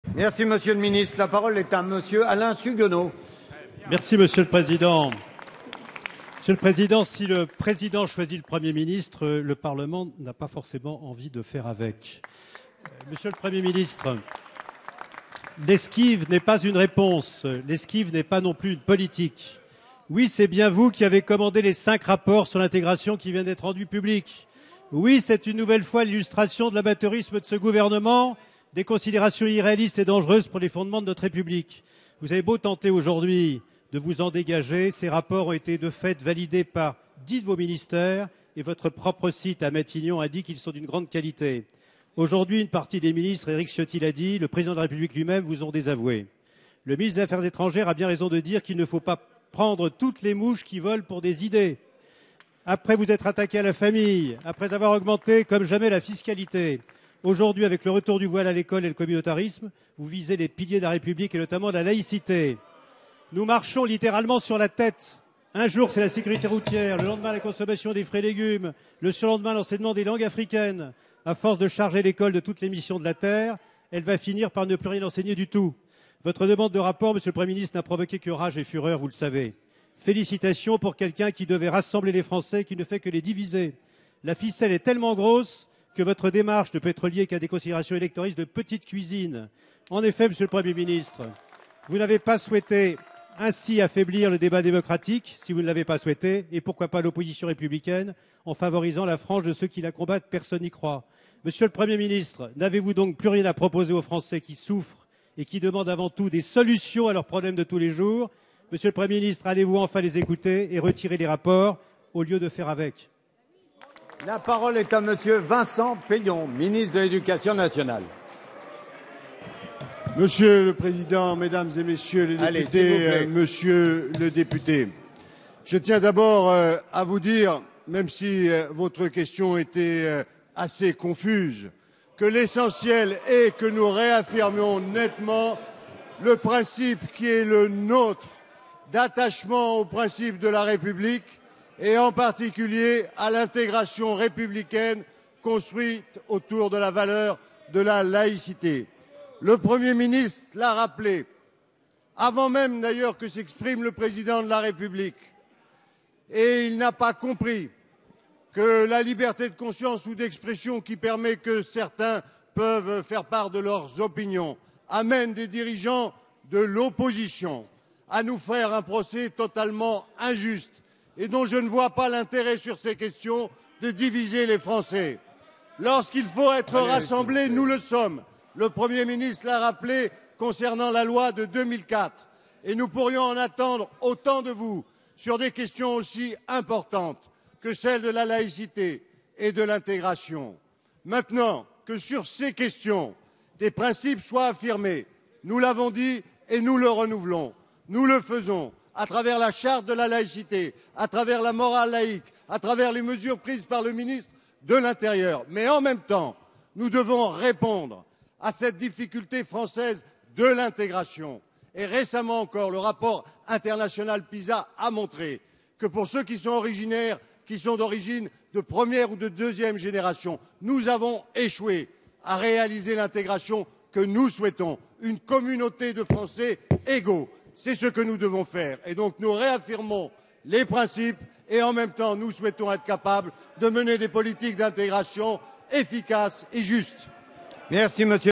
Alain Suguenot - Rapport sur l'intégration
Question d'Alain Suguenot, Député de Côte-d'Or, au sujet du rapport sur l'intégration cantonales (QAG - 18 décembre 2013)